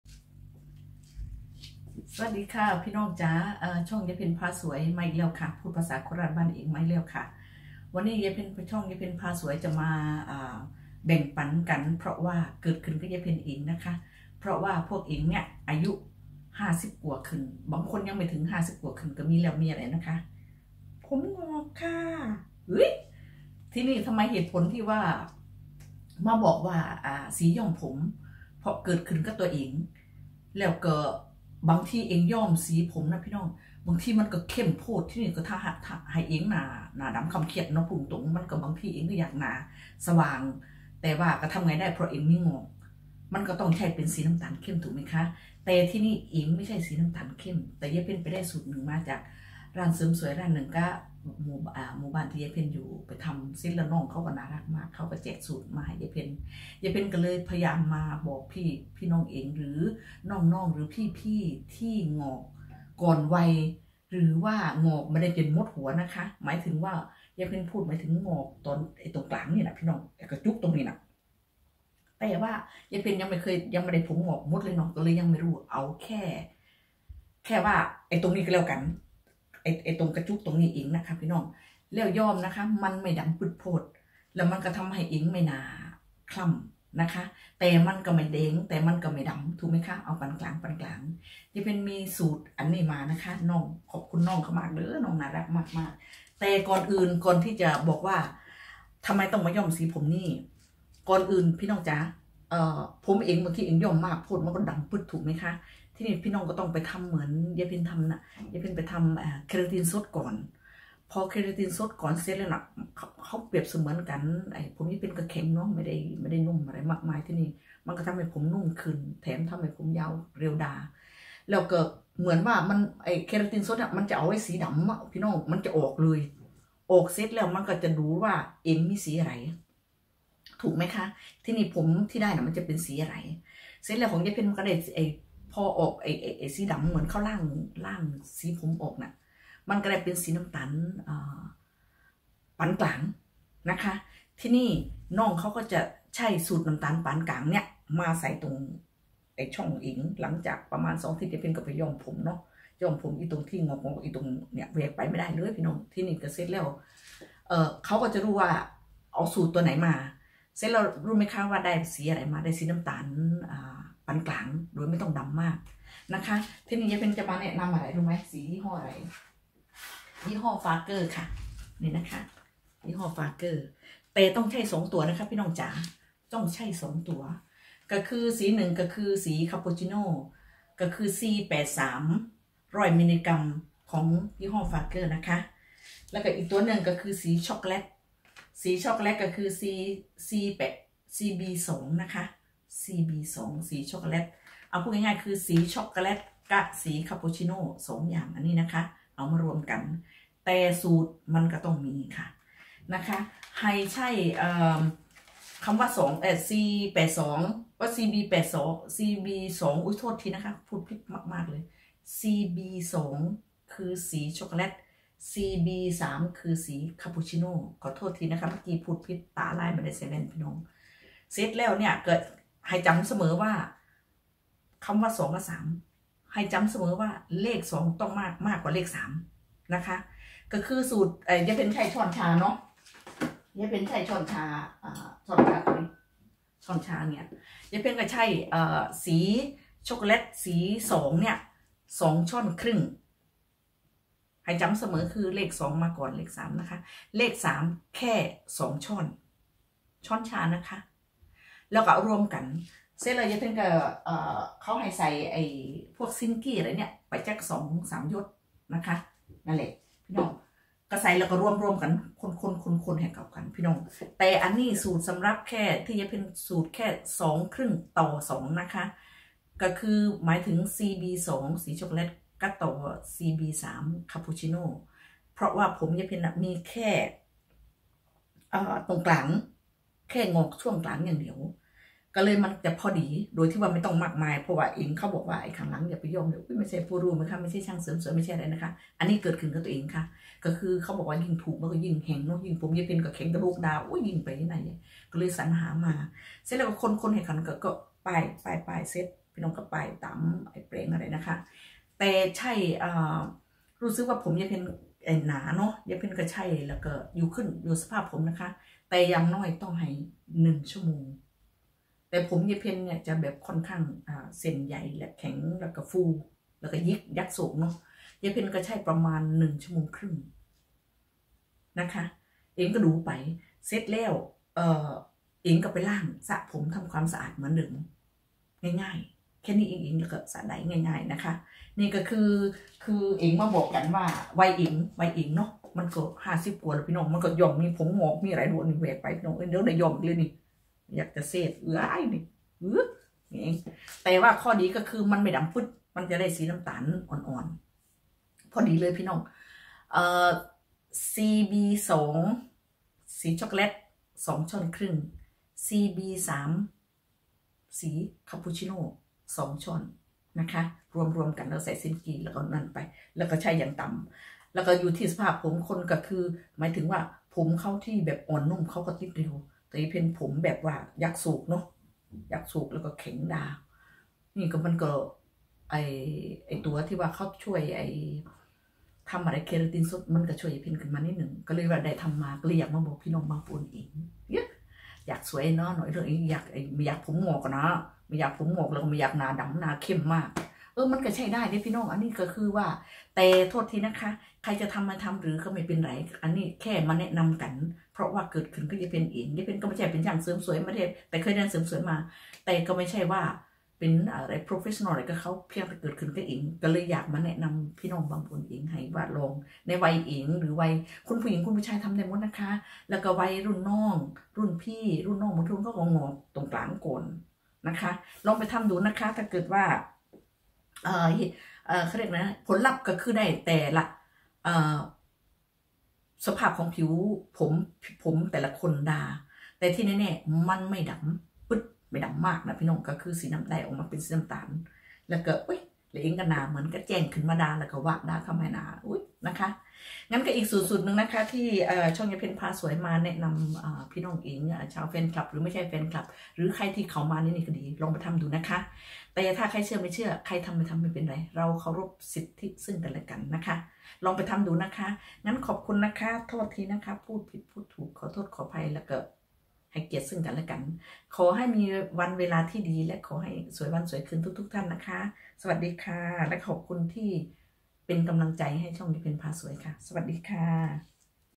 สูตรสีย้อมผมหงอกย้อมเองแบบง่ายๆ ที่บ้าน ผลลัพธ์คือสีน้าตาล หน้าสว่าง ดูสาวและสวยขี้นน้ะค้ะ😊
สียี่ห้อ
Ferger CB 2 ช็อกโกแลต  2 1/2 ช้อนชา
Ferger CB 3 คาโปชิโน 2 ช้อนชา
และน้ามันซิลกี้ 2 หยด

ผสมรวมกันและย้อม ใช้เวลา ประมาณ 1-1 1/2 ชม

ผลลัพธ์ที่ได้ขึ้นอยู่กับสภาพผมของแต่ละคนค่ะ😊